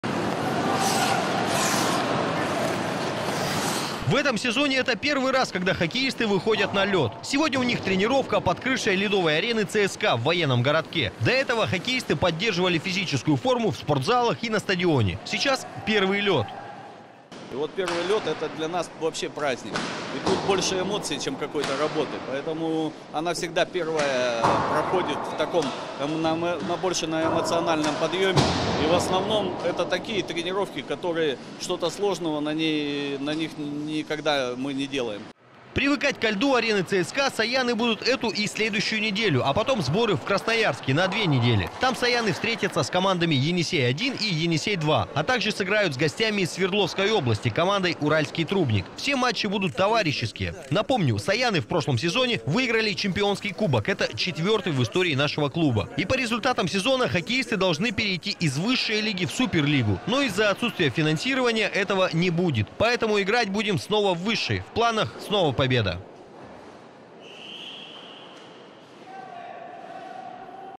«Саяны» провели первую тренировку на льду в Абакане
Коньки, клюшки, температура +10 и лёд. Наша хоккейная команда «Саяны» сегодня вышла на первую тренировку на льду. Это значит сезон ледовых баталий начался.